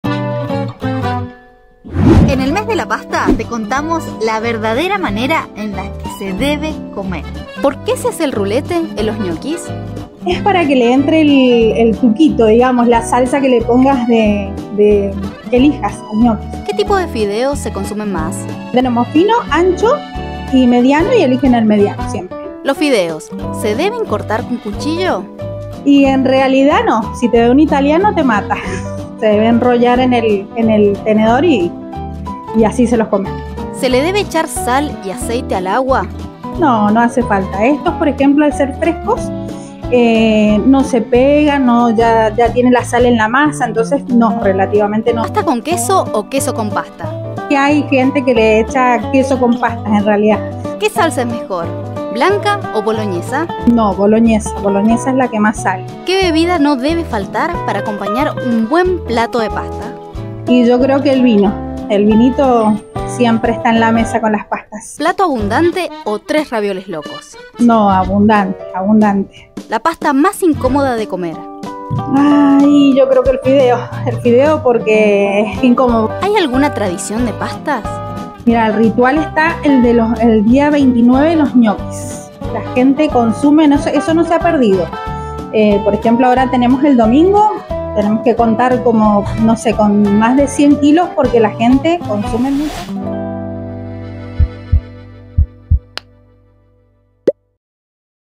🍝La VERDADERA manera de COMER las PASTAS
Se celebra en Día de la Pasta en todo el mundo. Con la llegada de este día nos sacamos las dudas de cómo se deben comer verdaderamente la pastas.